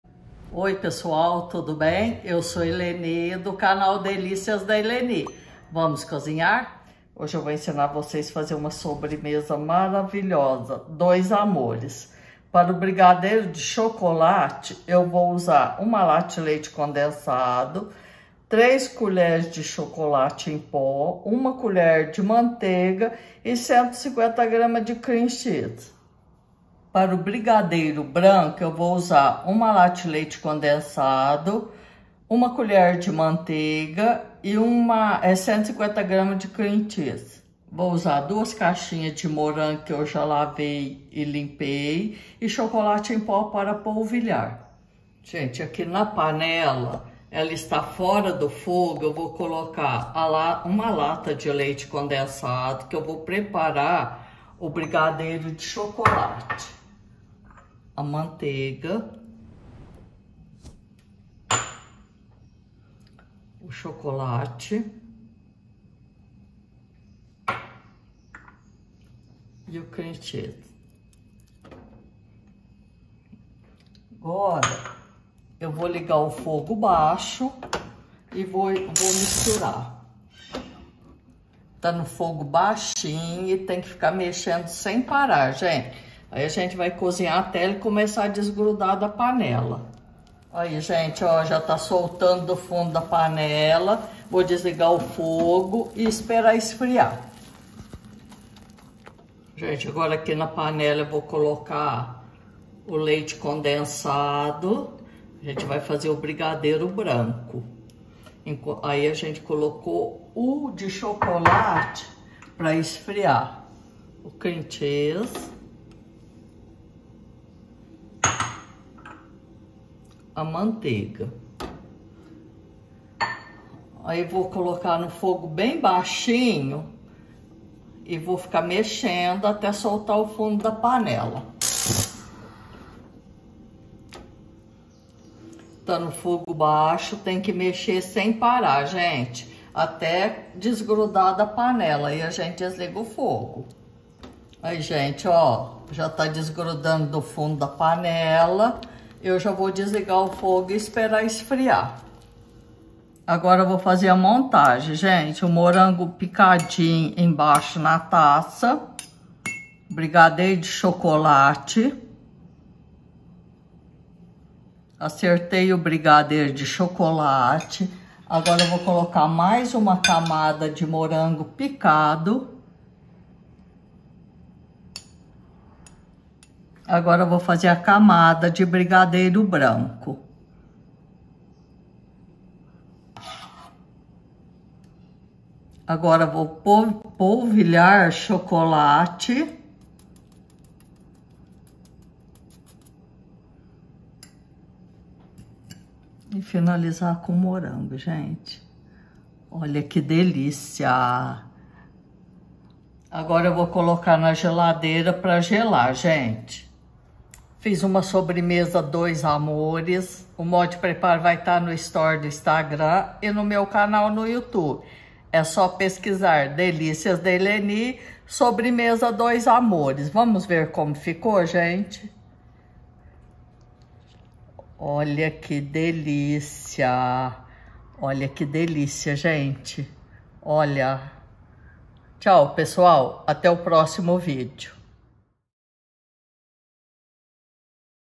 Sobremesa Dois Amores | Delícias da Eleni
Aqui no canal novas receitas todos os dias!

Essa sobremesa é muito fácil de fazer, além de rápida e saborosa!

Dois Amores

Brigadeiro de Chocolate

01 Lata de Leite Condensado;

03 Colheres (sopa) Chocolate em Pó;

01 Colher (sopa) Manteiga;

150 Gramas Cream Cheese;

Brigadeiro Branco

01 Lata de Leite Condensado;

01 Colher (sopa) Manteiga;

150 Gramas Cream Cheese;

Montagem

02 Caixinhas de Morango;

Chocolate em Pó a Gosto;